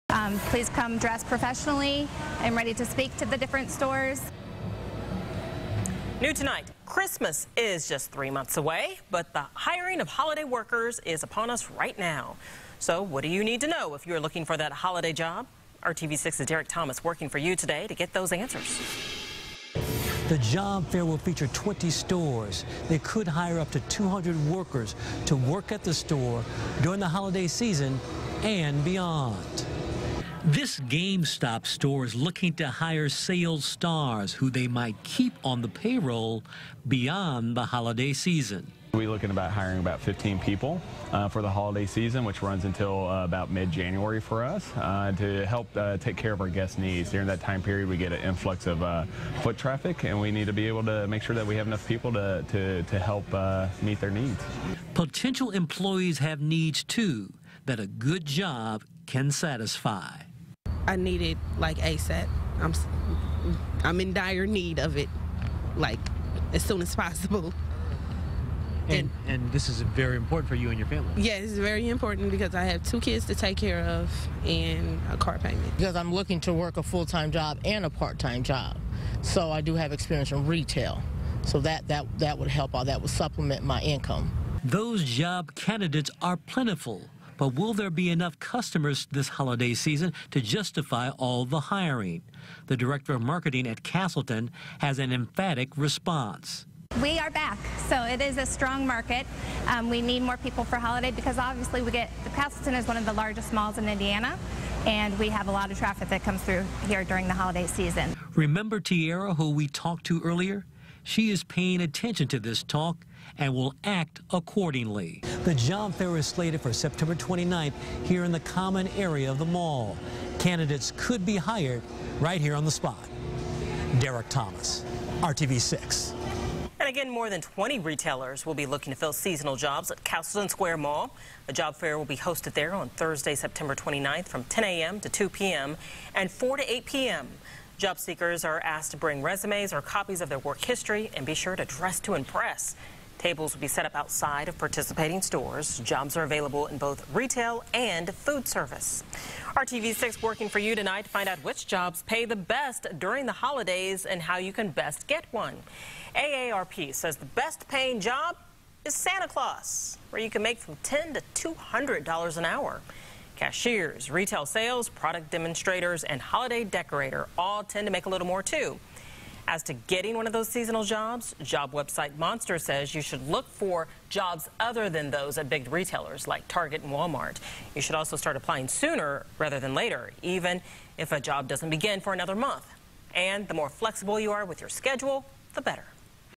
Castleton mall hosting job fair for seasonal positions
Dozens of jobs available for the holiday season and beyond.